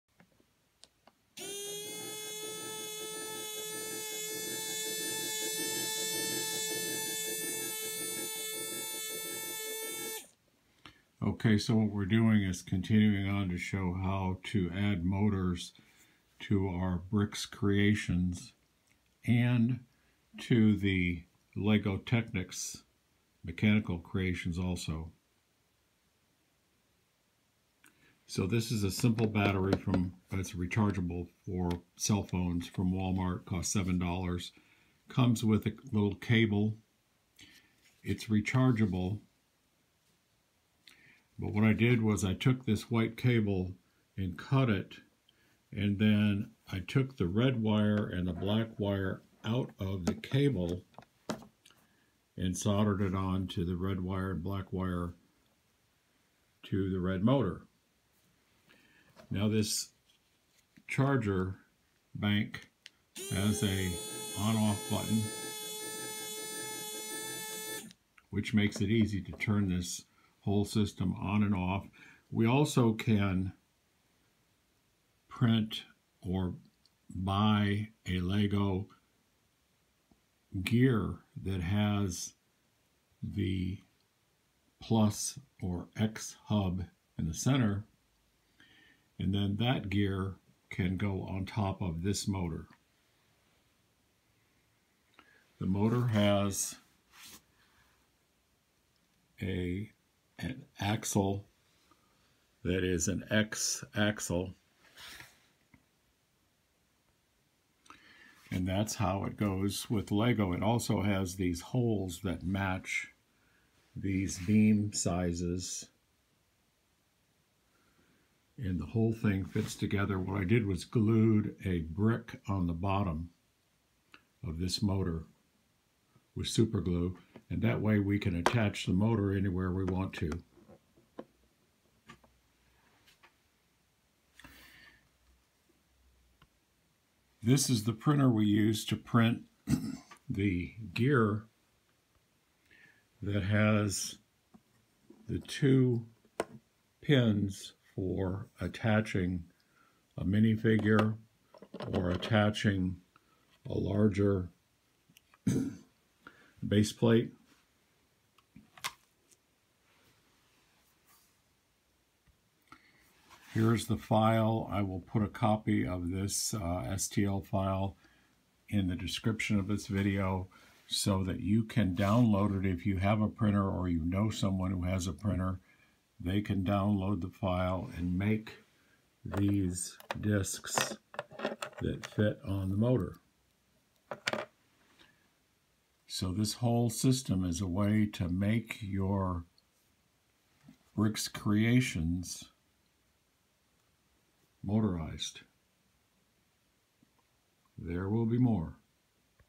LEGO Motorize Creations, Simple Way
Using Geekservo 3.7V DC motor and rechargeable battery bank. Cut any USB cable and find the red wire and black wire inside. Twist or solder these wires onto the red and black motor wires. Add a brick under the motor so it will snap onto a LEGO base plate.  The below link is a free file that you can print which has the X or plus hole in the disc with two studs for mini figures or other creations.